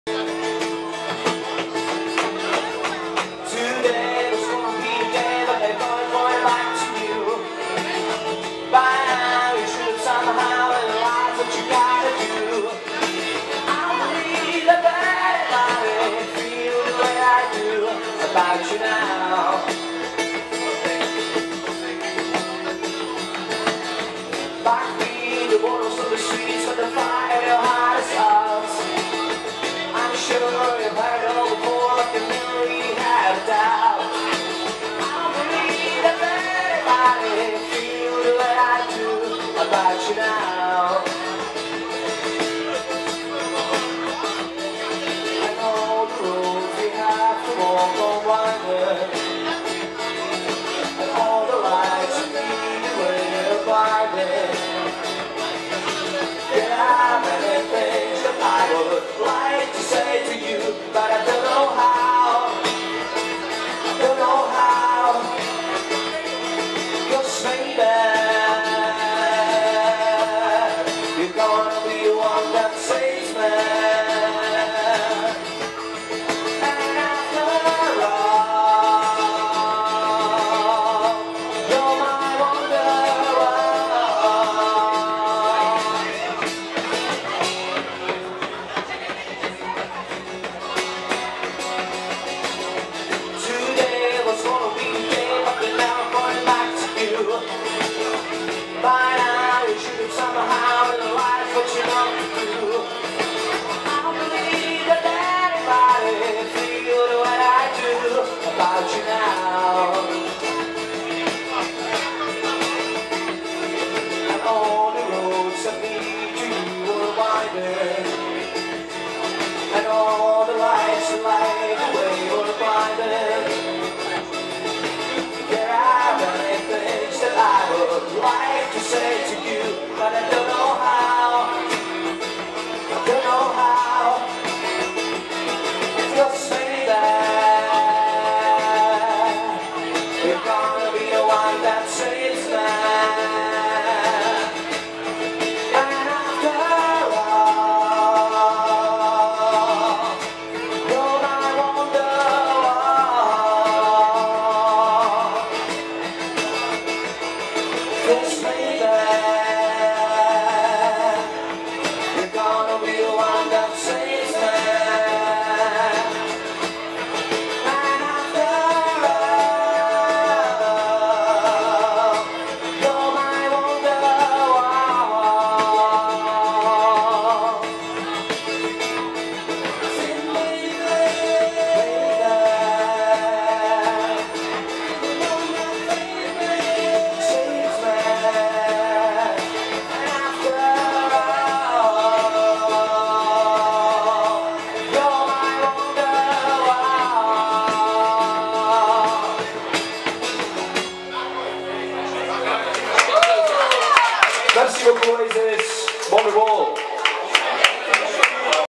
Flynn - Live Bol d'Air 3/9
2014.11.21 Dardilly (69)
Flynn à Bol d'Air, belle soirée aux tonalités folk et rock.